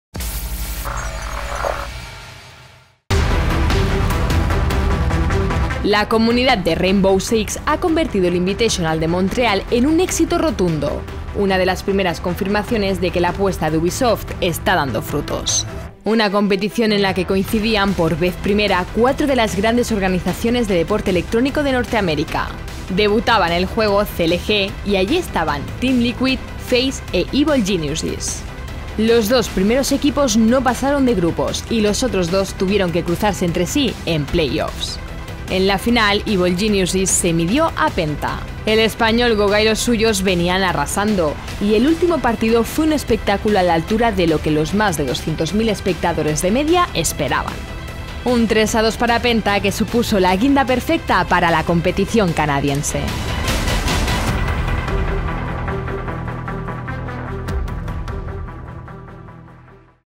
Repasamos el Six Invitational de Montreal
El pasado fin de semana tuvo lugar el Rainbow Six Siege: Six Invitational en Montreal, en el que se dieron cita los mejores equipos del panorama internacional, y en el que el español Goga volvió a brillar con los suyos.